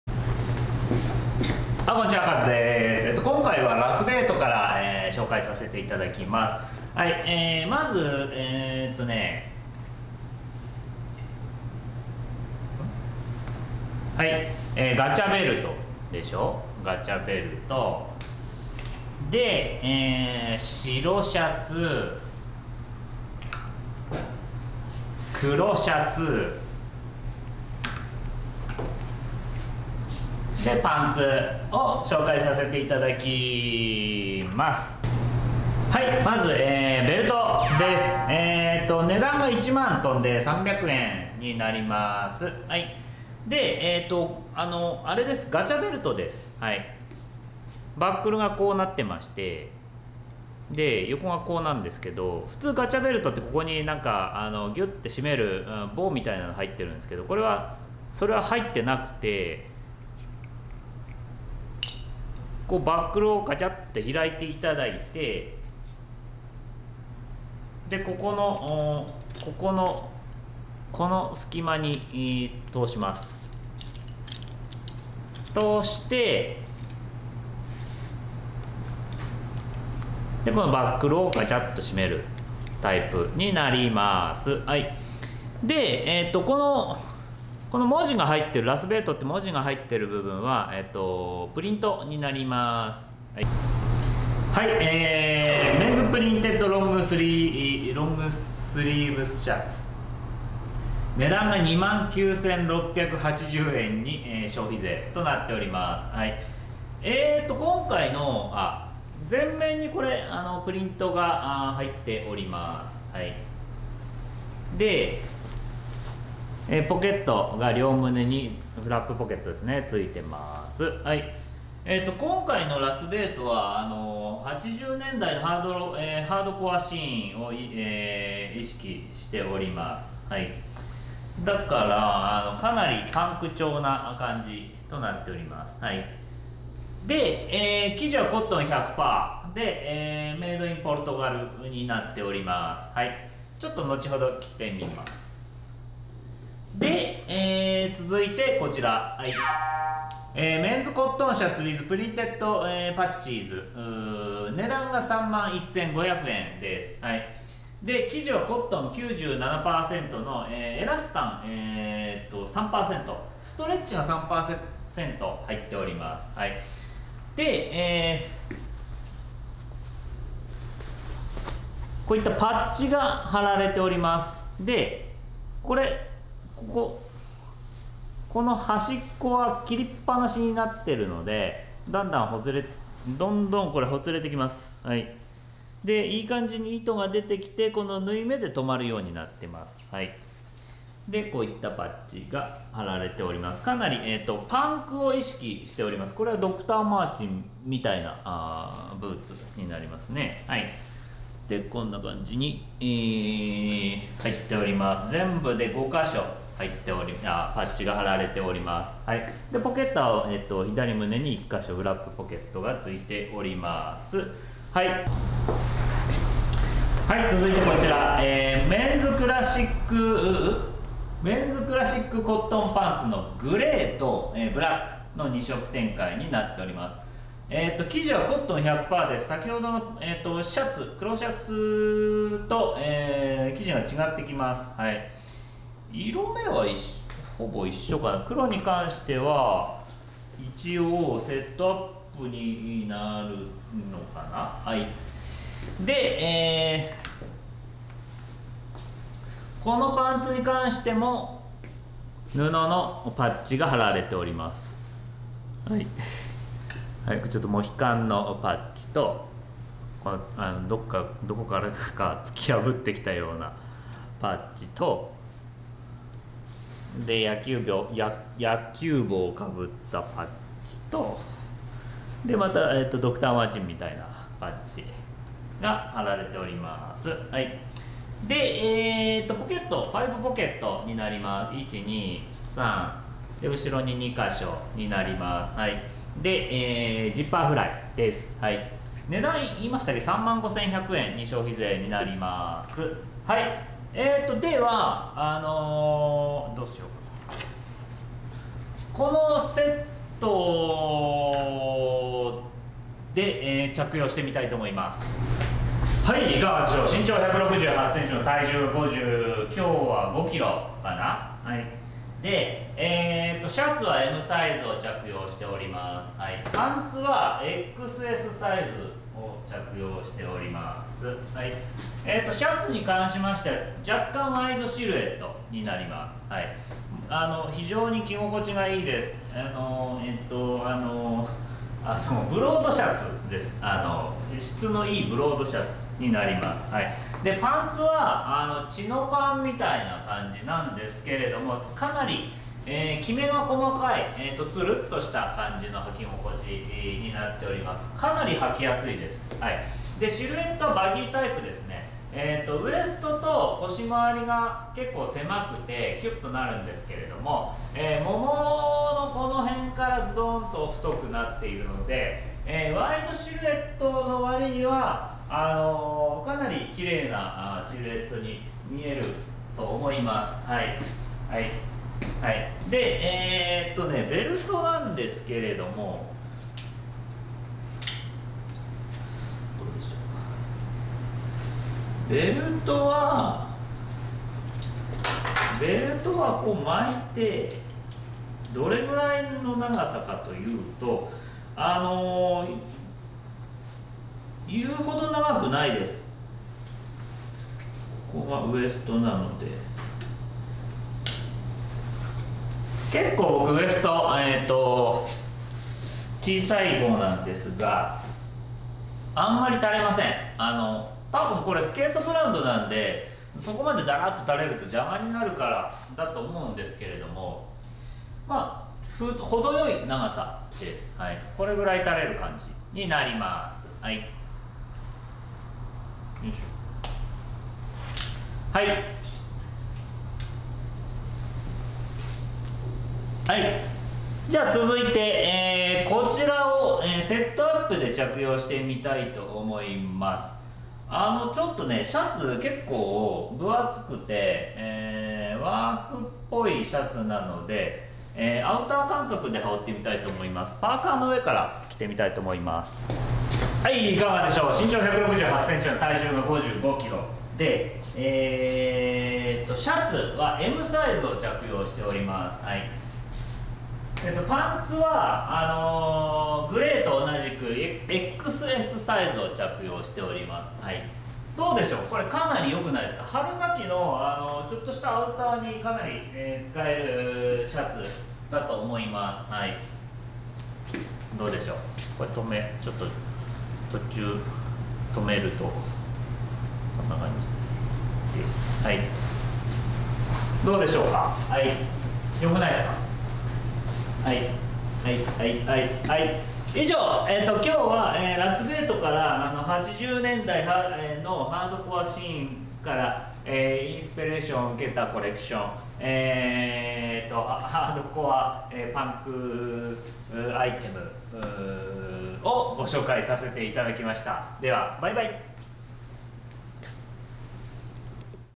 80年代のハードコアシーンと身体美にインスピレーションを受けたパンクなアイテム【ラスベートSS21】
RASSVET
Men's Printed Webbing Belt

Men's Cotton Classic Pants
Grey